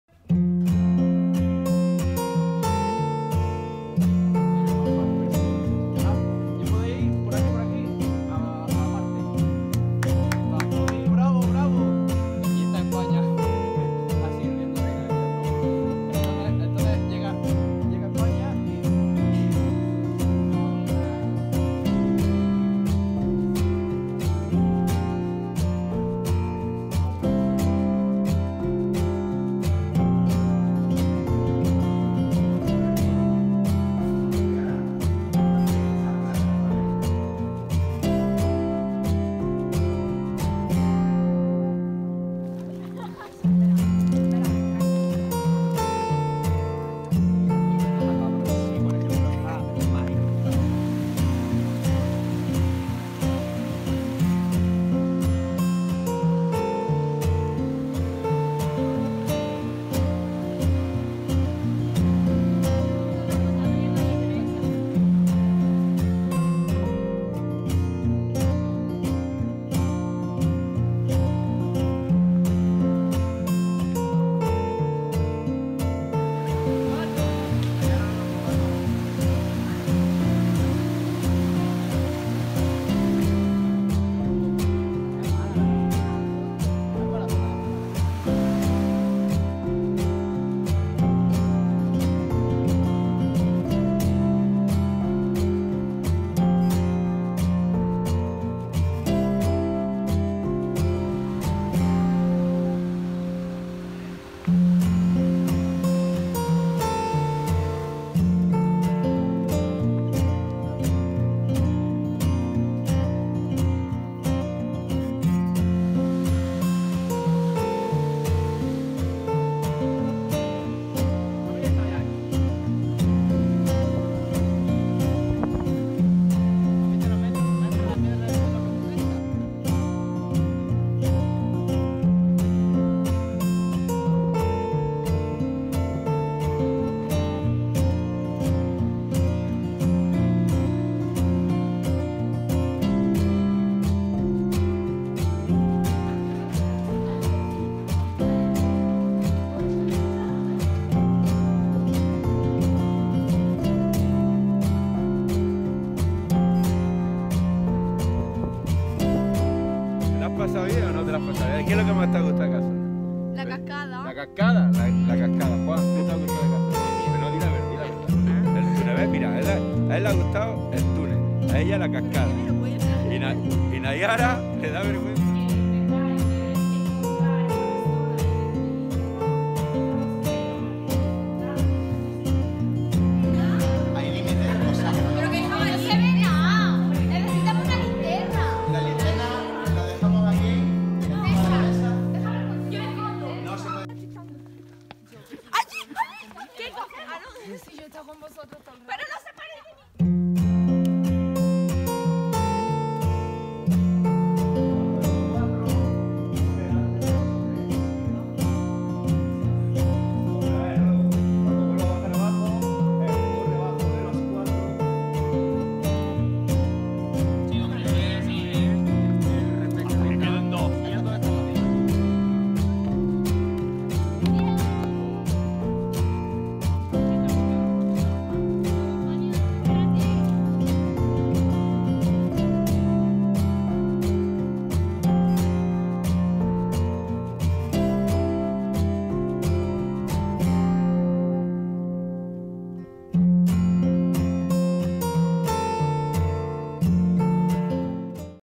"Cazorla" Sierra Viva e Infinita
Estancia del alumnado de 1º Bachillerato y Ciclo Formativo en el Aula de la Naturaleza del Cantalar en pleno corazón del Parque Natural de las Sierras de Cazorla, Segura y Las Villas.